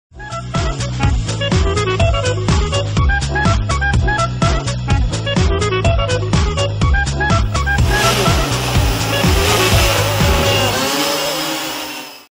Animace hexacoptera
Tvorba animace hexacoptery. vytvořil jsem 3D model hexacoptery, definoval mu parametry pohybu včetně rotace vrtulek a použil jsem ho v malém promo videu.